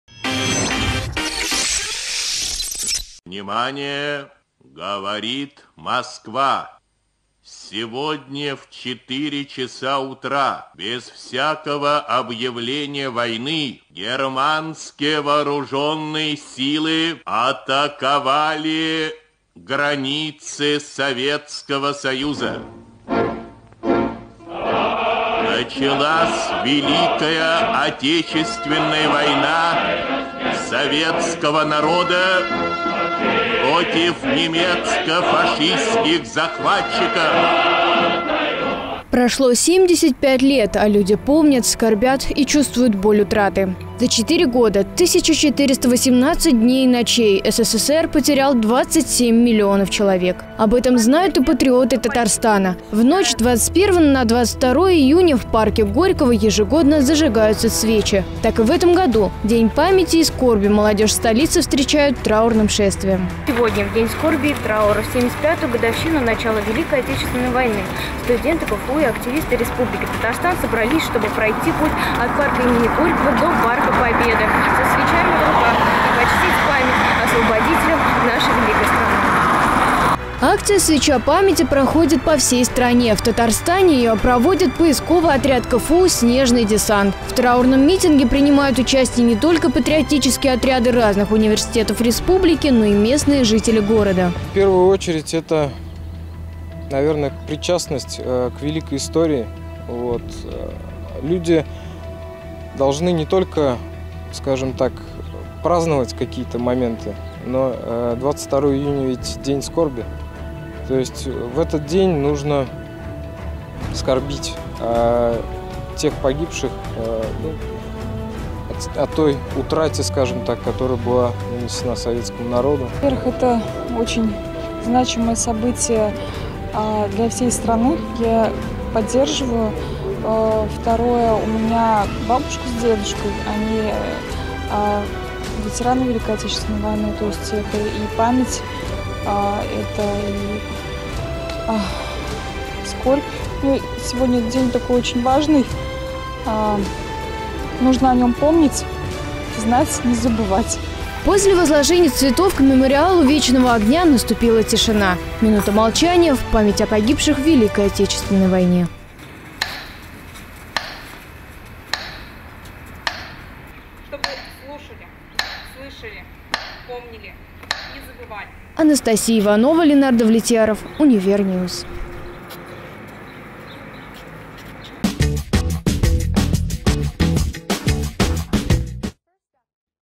День памяти и скорби от 22.06.2016
22 июня в России и странах СНГ отмечается День памяти и скорби в ознаменование годовщины начала Великой Отечественной войны. В 4 часа утра началась страшная война и вот, спустя 75 лет вся страна, а в том числе, молодежь Казани встречает рассвет под мирным небом.